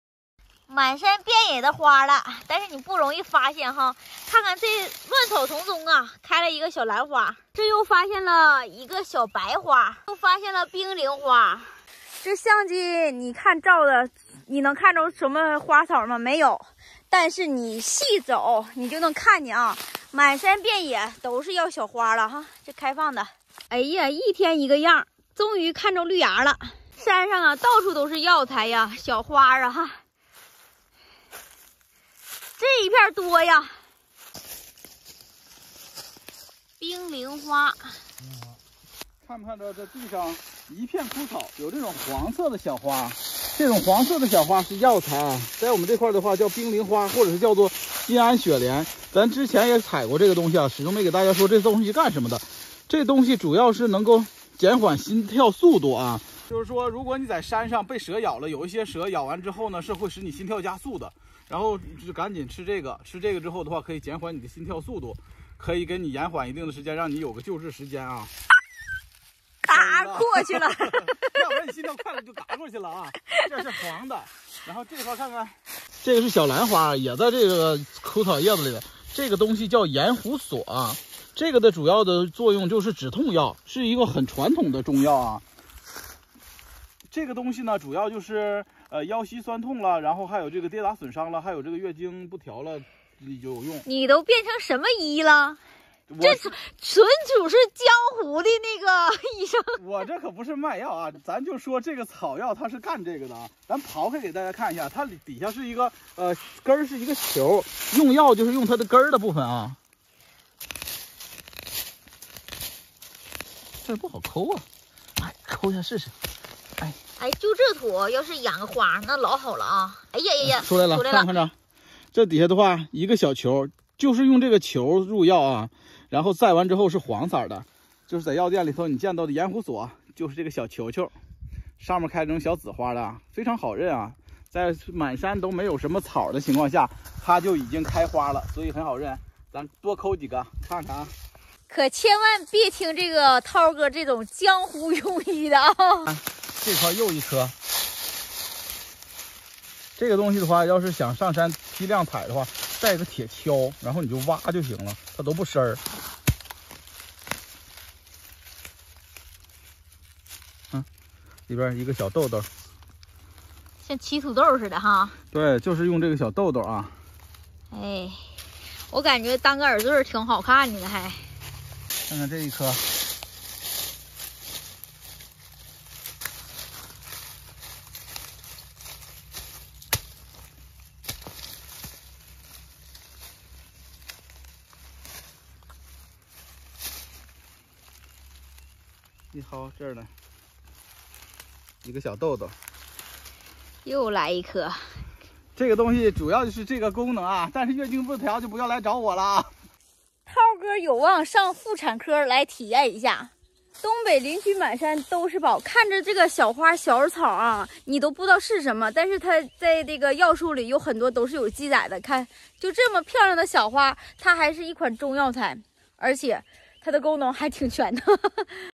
東北林區滿山都是藥材，內向的老公上山話就多，藥材講的頭頭是道！ 【雨奈何雲】
我们的视频旨在传授合法的害虫控制技巧，保障家庭健康，不会对动物造成伤害。 東北林區滿山都是藥材，內向的老公上山話就多，藥材講的頭頭是道！ 【雨奈何雲】